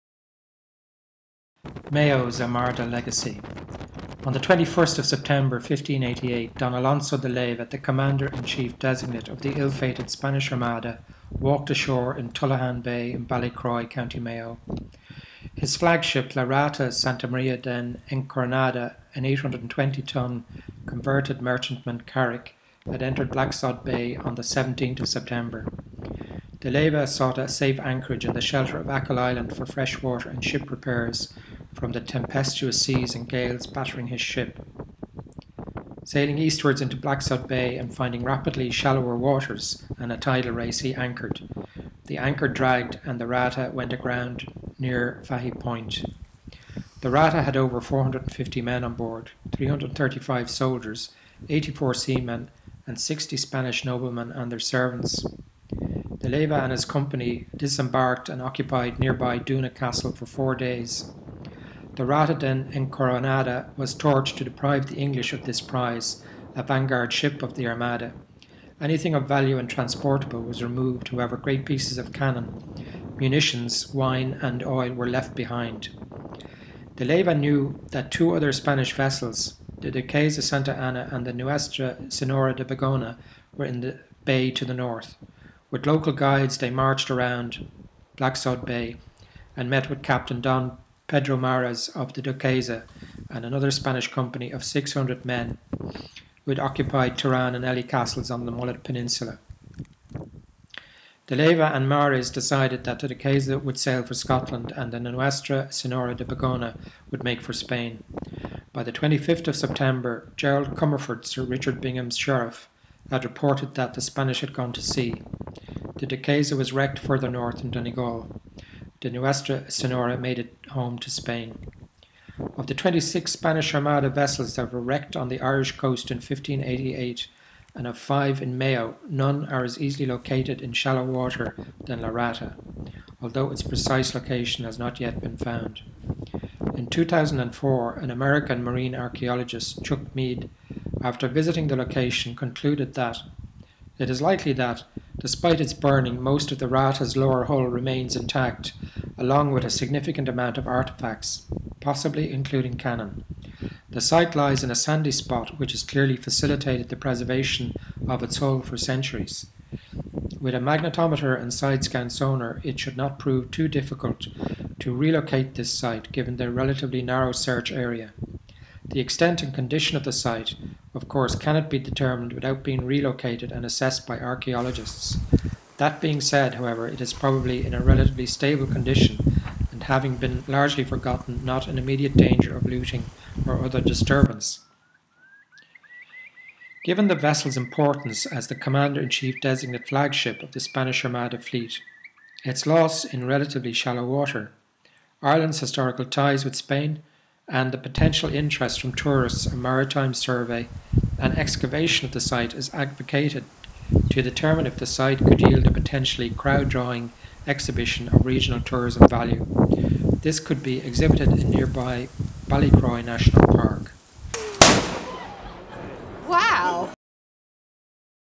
La Rata 17092013
Mayo's Armada legacy

On the 21st. of September 1588 Don Alonso de Leyva,  the Commander-in-chief designate of the ill fated Spanish Armada, walked ashore in Tullaghan Bay in Ballycroy, County Mayo.

His flagship La Rata Santa Maria de Encoronada, an 820 ton converted merchantman carrack, had entered Blacksod Bay on the 17th September. De Leyva sought  a safe anchorage in the shelter of Achill island for freshwater and ship repairs from the tempestous seas and gales. Sailing eastwards into Blacksod Bay and finding rapidly shallower waters and a tidal race he anchored. The anchor dragged and the Rata went aground near Fahy Point.

The Rata had over 450 men onboard, 335 soldiers, 84 seamen, sixty Spanish noblemen and their servants. De Leyva and his company disembarked and occupied nearby Doona castle for four days. The Rata de Encoronada was torched to deprive the English of this prize, a vanguard flagship of the Armada. Anything of value and transportable was removed however great pieces of canon, munitions, wine and oil were left behind.

De Leyva knew that two other Spanish vessels, the Duquesa Santa Ana and the Nuestra Senora de Begona were in the bay to the north. With local guides they marched around the bay and met with Captain Don Pedro Mares of the Duquesa and another Spanish company of 600 men at Tiraun and Elly castles on the Mullet peninsula.

De Leyva and Mares decided the Duquesa would sail for Scotland and the Nuestra Senora de Begona would make for Spain. By the 25th September Gerald Comerford Sir Richard Binghams Sheriff had reported, that the Spanish had gone to sea. The Duquesa was wrecked further north in Donegal. The Nuestra Senora made it home to Spain.

Of the 26 Spanish Armada vessels that were wrecked on the Irish coast in 1588 and of five in Mayo, none are as easily located in shallow water than La Rata, although its precise location has not yet been found.

In 2004 American marine archaeologist Chuck Meide after visiting the location concluded that,"It is likely that, despite its burning, most of the Rata's lower hull remains intact, along with a significant amount of artefacts (possibly including cannon). The site lies in a sandy spot which has clearly facilitated the preservation of its hull for centuries.
With a magnetometre and side scan sonar, it should not prove too difficult to relocate this site, given the relatively narrow search area. The extent and condition of the site, of course, cannot be determined without being relocated and assessed by archaeologists. That being said, however, it is probably in a relatively stable condition and, having been largely forgotten, not in immediate danger of looting or other disturbance."
Given the vessels importance as the Commander in chief designate flagship of the Spanish Armada fleet, its loss in relatively shallow water, Ireland's historical ties with Spain and the potential interest from tourists a marine survey is advocated to assess if an excavation of the site could yield a potentially crowd drawing exhibition of regional tourism value.

Ireland National Monuments Service set up in 2000 an Underwater Archaeological Unit to manage and protect its underwater cultural heritage. Recently they carried out a preliminary survey and are due to revisit the site in 2013.